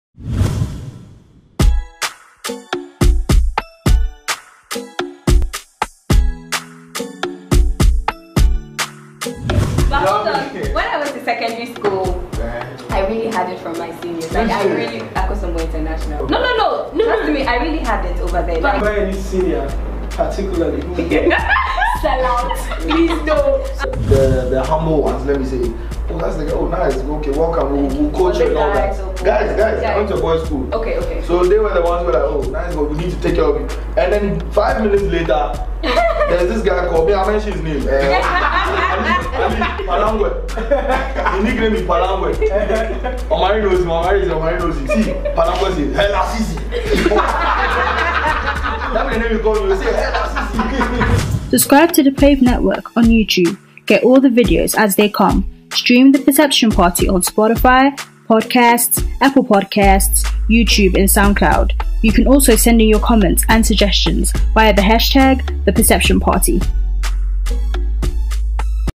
CYRIL OF YOLO FAME AND NINA OF HOME SWEET HOME GUESTS ON THE PERCEPTION PARTY!
GROWING UP AS CHILD STARS IN GHANA